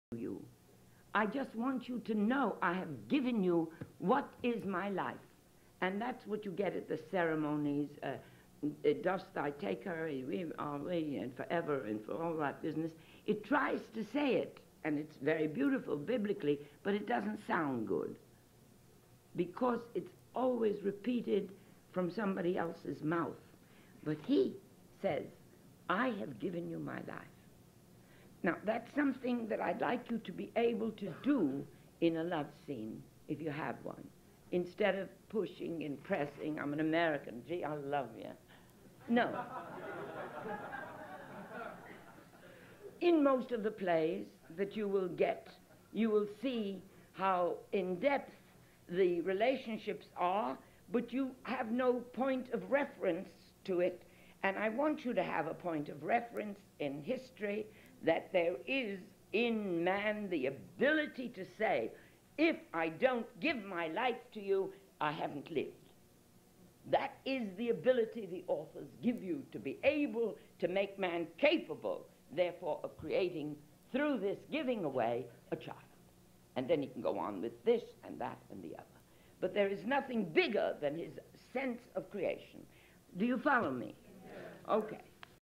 Stella • Love and Depth
A running theme of Stella's was that we were never finding the depth of ideas, their size. Here she takes love and romance and lifts the idea.